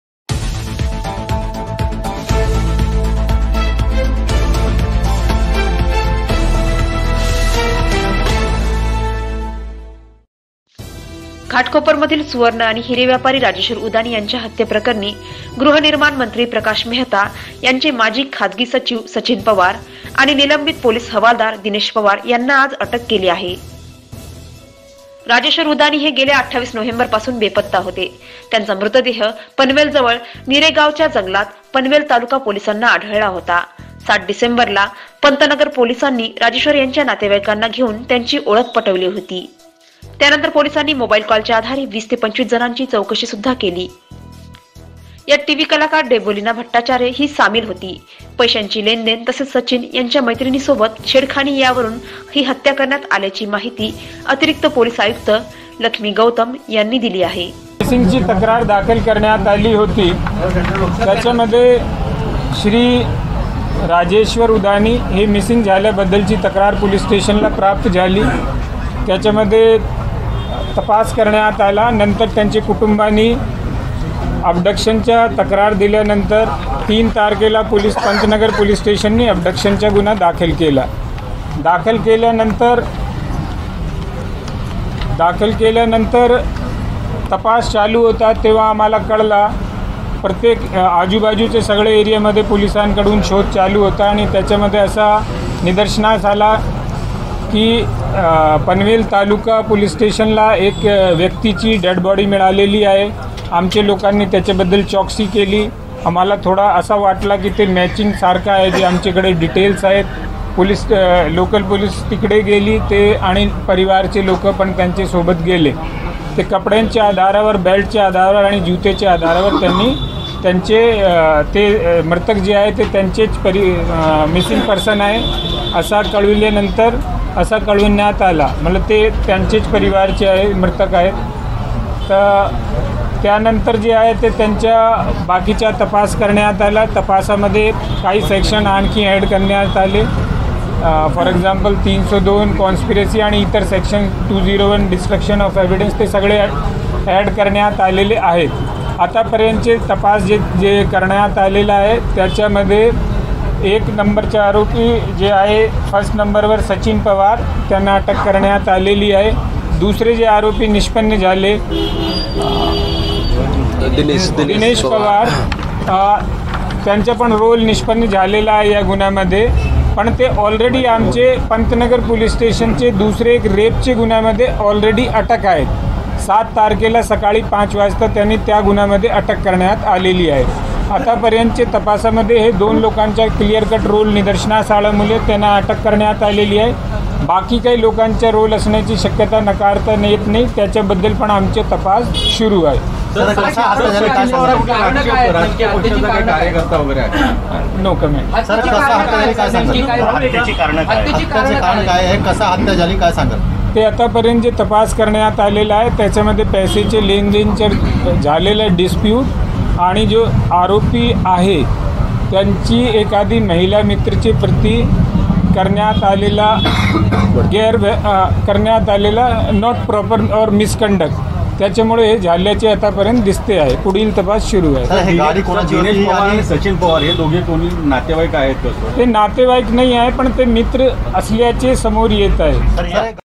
या दोन कारणाने केली हिरे व्यापारी राजेश्वर उदानी यांची हत्या ?
मुंबई - घाटकोपर मधील सुवर्ण आणि हिरे व्यापारी राजेश्वर उदानी यांची हत्या गृहनिर्माण मंत्री  प्रकाश मेहता यांचा माजी खासगी सचिव सचिन पवार आणि निलंबित पोलीस हवालदार दिनेश पवार यांना अटक केली आहे.राजेश्वर उदानी हे गेल्या 28 नोव्हेंबर पासून बेपत्ता होते.त्यांचा मृतदेह पनवेल जवळ निरे गावाच्या जंगलात पनवेल तालुका पोलिसना आढळला होता.7 डिसेंम्बरला पंतनगर पोलिसांनी राजेश्वर यांच्या नातेवाईकांना घेऊन त्यांची ओळख पटविली होती.त्यानंतर पोलिसांनी मोबाईल कॉलच्या आधारे 20 ते 25 जनांची चौकशी सुद्धा केली यात टीव्ही कलाकार देबोलीन भट्टाचार्य ही सामील होती.पैश्याची लेनदेन तसेच सचिन याच्या मैत्रिणीसोबत छेडखनी यावरून हत्या करण्यात आल्याची माहिती अतिरिक्त पोलीस आयुक्त लखमी गौतम यांनी दिली आहे.